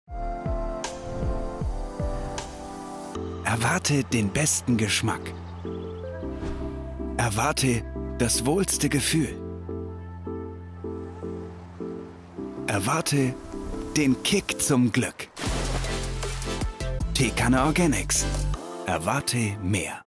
Teekanne Organics. Erwartee mehr.
Bio-Tee in seiner schönsten Form – die Teekanne ORGANICS
Jung, frech und so lecker – das sind unsere Bio-Tees TEEKANNE ORGANICS. Mit einem motivierenden Spruch auf den Lippen und purem Geschmack begleiten sie Dich durch den Tag.

Ganze vorne auf ihrer Fahne steht: Bio-zertifiziert! Umweltbewusstsein liegt uns am Teeherzen, deswegen haben wir diese zehn Sorten kreiert, die diesen Gedanken bis ins kleinste Teeblatt tragen. Alle Zutaten stammen ausschließlich aus Anbauten, an denen die Tees, Früchte und Kräuter auf ganz natürliche Weise wachsen und gedeihen – und das schmeckt man! So verweilen sie in ihren Teebeuteln und der umweltfreundlichen Verpackung. Was hinter dem schicken Design auf der Faltschachtel steckt, ist FSC-Papier, also Papier, das getreu des Forest Stewardship Councils® umweltfreundlich und ressourcenschonend hergestellt wurde. Bei unseren Tees stimmen sowohl die inneren als auch die äußeren Werte.